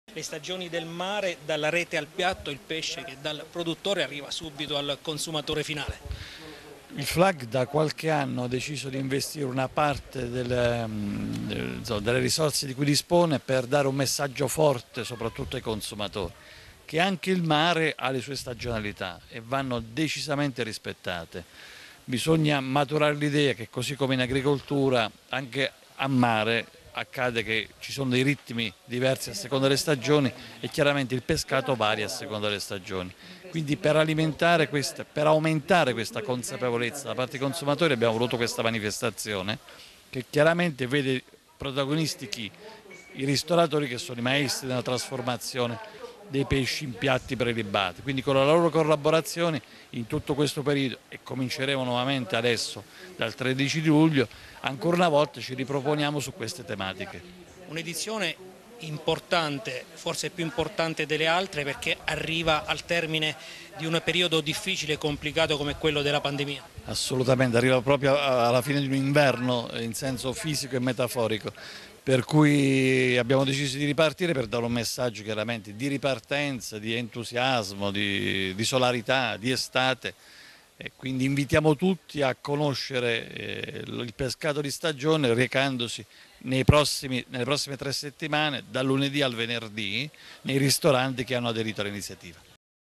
Torna le “Stagioni del Mare” a Rocca San Giovanni
Presentata questa mattina a Rocca San Giovanni l’edizione 2021 de “Le Stagioni del Mare”: dalla Rete al Piatto, organizzata dal Flag Costa dei Trabocchi